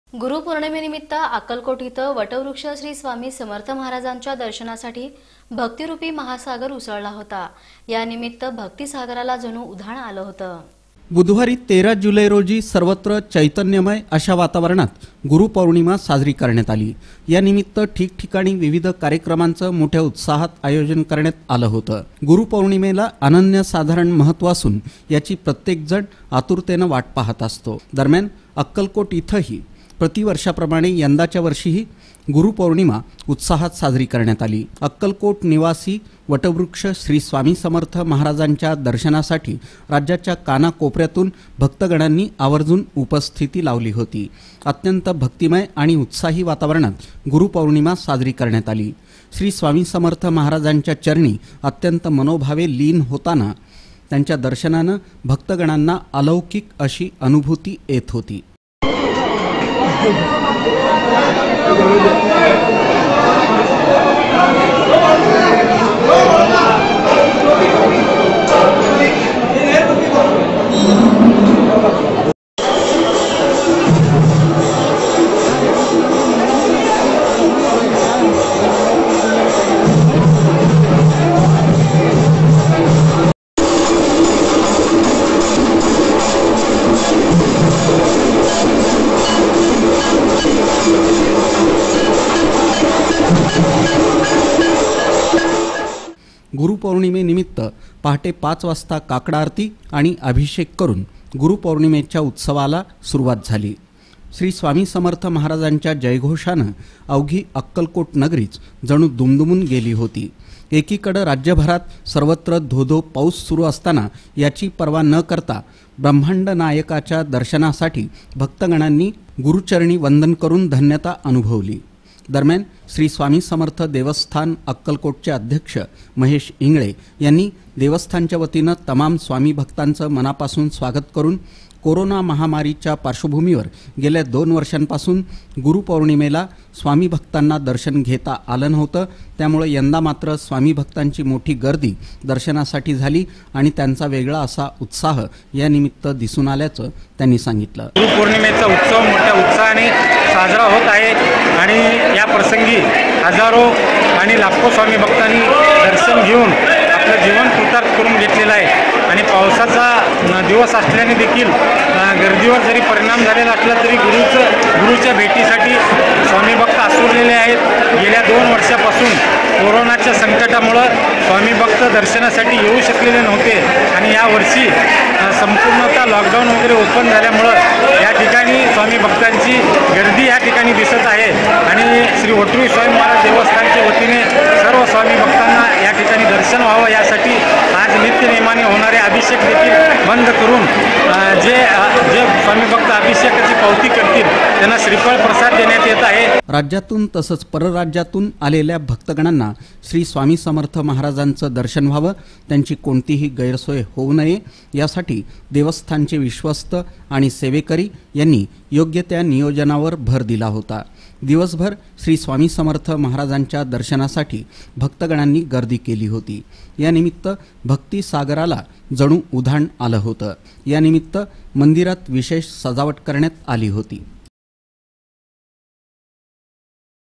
अक्कलकोटमध्ये गुरु पौर्णिमा साजरी... दर्शनासाठी उसळला भक्तीरूपी महासागर... | AKKALKOT GURU POORNIMA
अक्कलकोटमध्ये गुरु पौर्णिमा साजरी... दर्शनासाठी उसळला भक्तीरूपी महासागर...